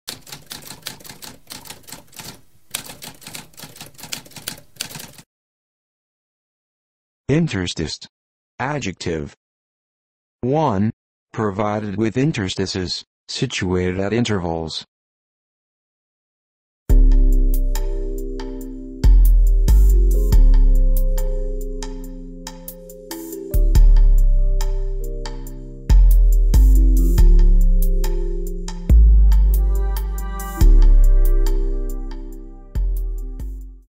What does intersticed mean?
A spoken definition of intersticed.

Intro Sound:
Typewriter - Tamskp